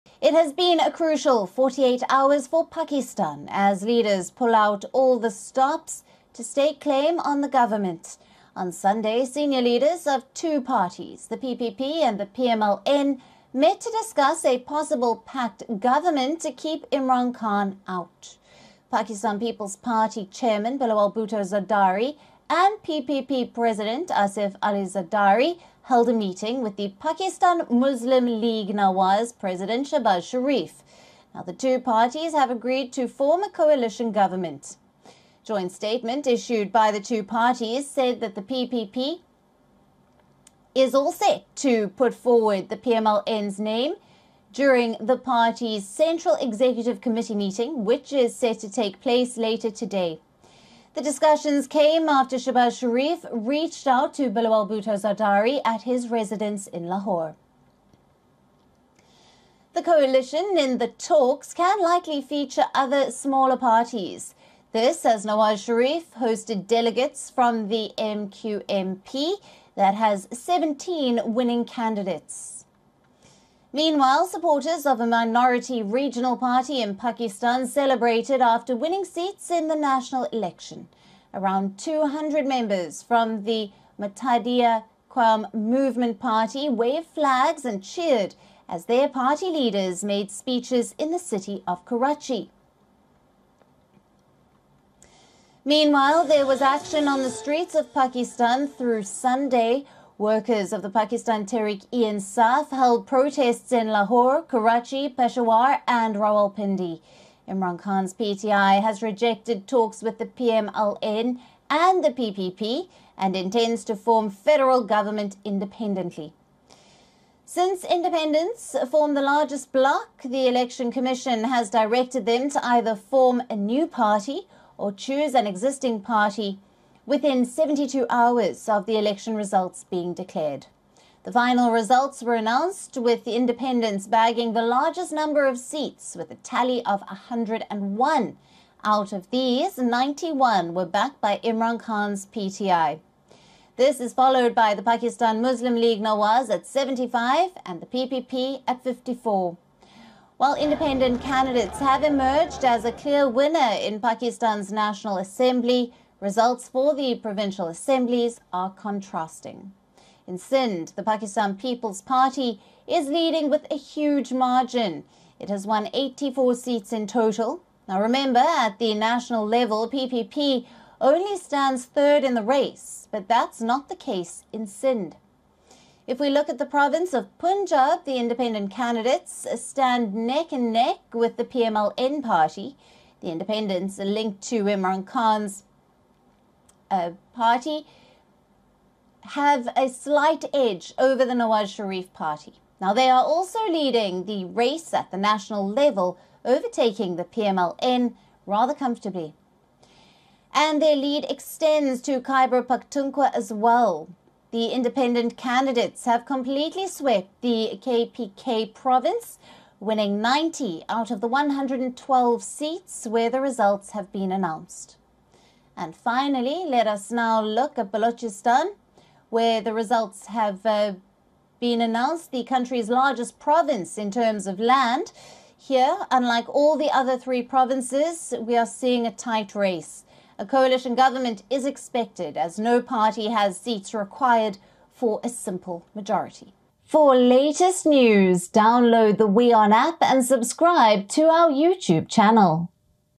Pakistan election results 2024: PPP & PML-N agree on political cooperation | WION
The hazy picture about who will form Pakistan's next government seems to be clearing out. Nawaz Sharif urges rival parties to join hands. Now in a major breakthrough, the Nawaz Sharif led PML-N and Bilawal Bhutto Zardari led PPP have in principle agreed on political cooperation. Nawaz Sharif has been reaching out to, MQM-P, JUI-F for coalition government. Watch to know more!